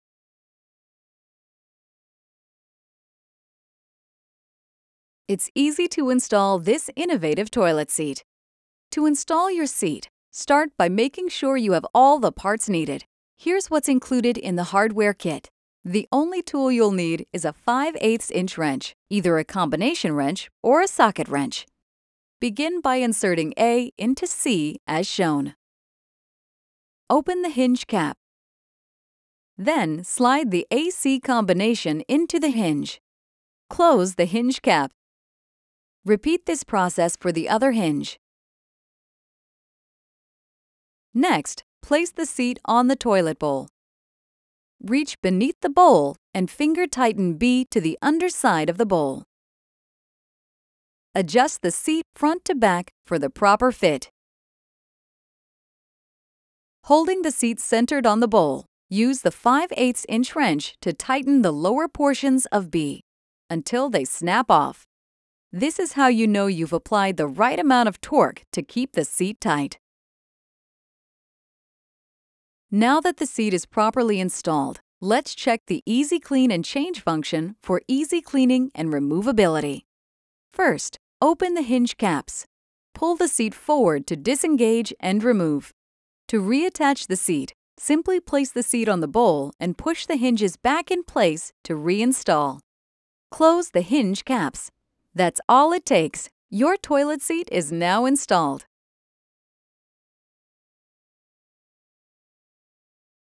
Bemis Affinity with E4 Installation
Learn how to install the Bemis Affinity toilet seat with E4 hinge technology.